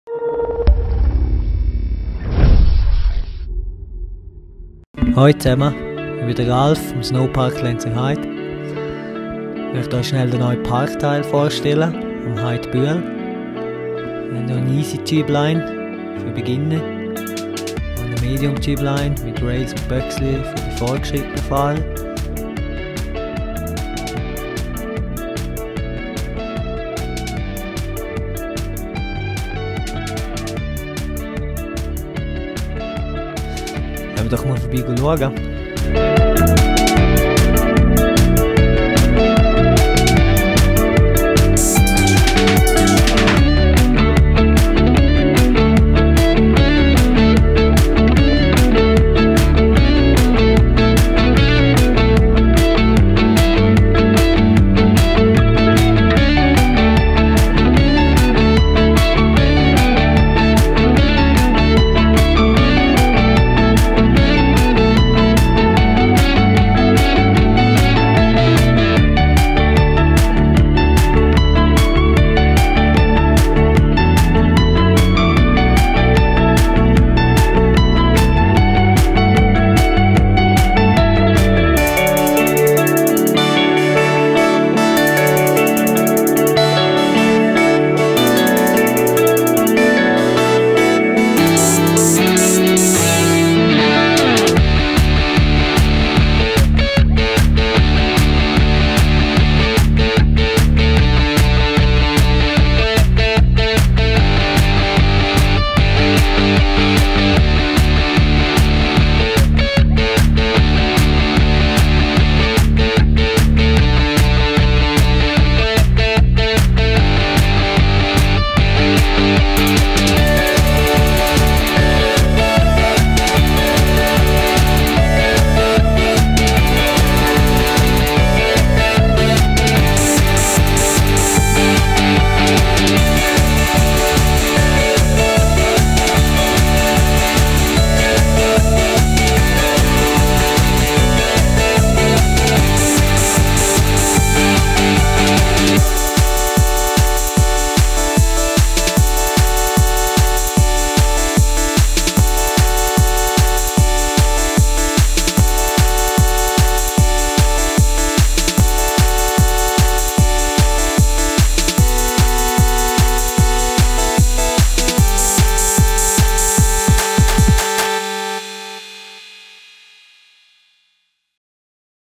Snowpark Lenzerheide: New Area & Slide Side Freeski Followlines - 06.02.2014
The session didn't leave much to the imagination: The day was blessed with sunshine, blue skies and the multitude of motivated shredders made the shoot at Snowpark Lenzerheide a pleasure to attend. Check the edit with the New Area and the Slide Side presented in some nice follow runs with some smooth tricks by Thiemo Simeon!
Check and qparks.com for the full report!